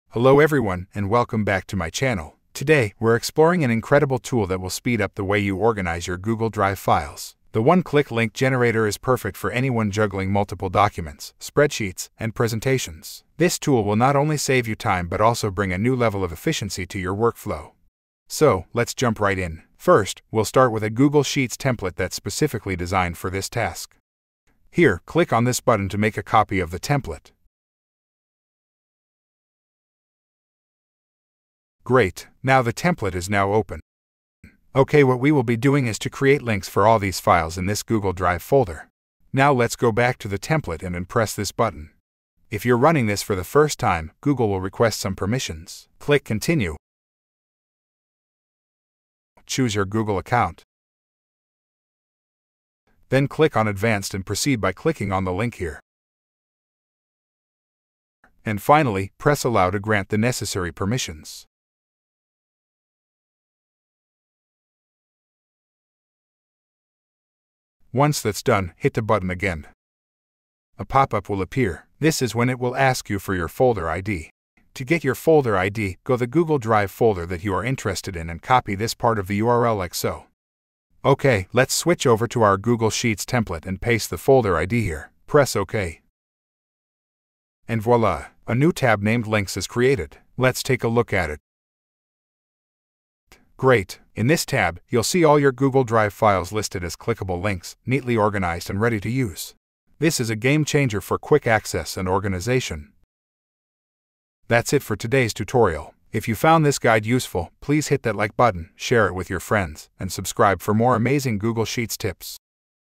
Instantly Create Links for All Files in Your Folder in One-Click!
Discover the simplicity of creating URL or hyperlinks for ALL your Google Drive files with just one click! 🌟

I can help you save time and simplify your tasks by creating custom automations and solutions.